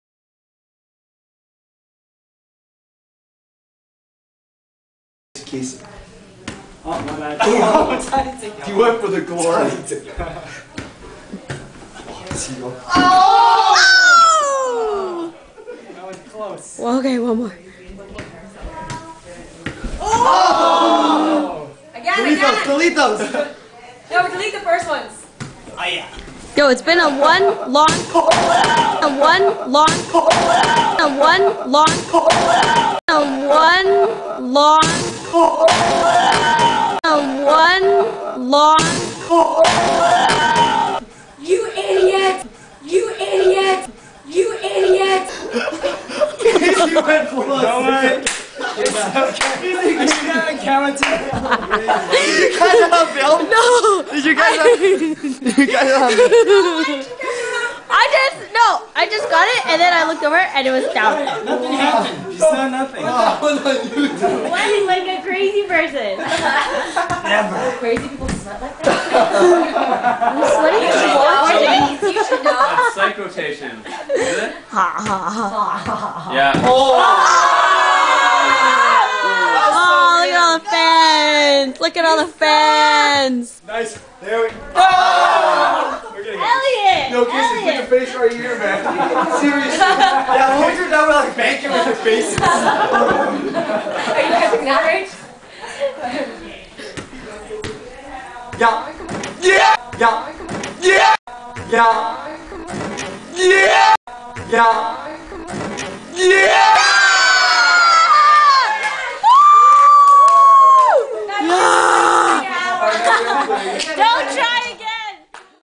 Shaolin Soccer @ Hermes'
this is what happens when you put three hyperactive boys together with a soccer ball